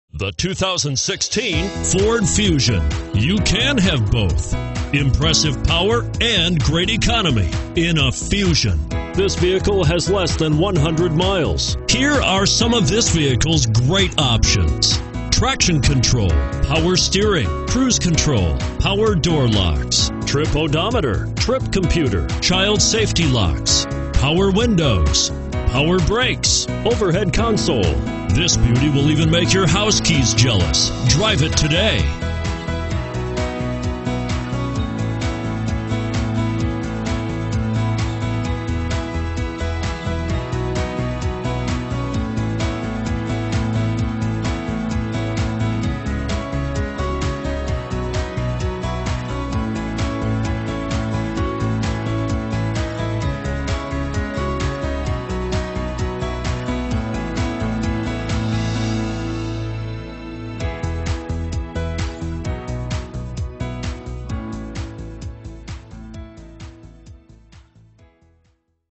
2016 Ford Fusion Frankfort, Chicago, Bolingbrook R3873
2016 Ford Fusion S

Steer your way toward stress-free driving with anti-lock brakes, a backup camera, traction control, side air bag system, and emergency brake assistance in this 2016 Ford Fusion S.  It has a 2.5 liter 4 Cylinder engine.  With an unbeatable 5-star crash test rating, this 4 dr sedan puts safety first.  The exterior is a beautiful ruby red metallic tinted clearcoat and the interior is a lovely earth gray.  With Bluetooth, you can access your electronics anywhere, anytime.  You won't believe the amazing sound quality from this vehicle's CD player and an MP3 player.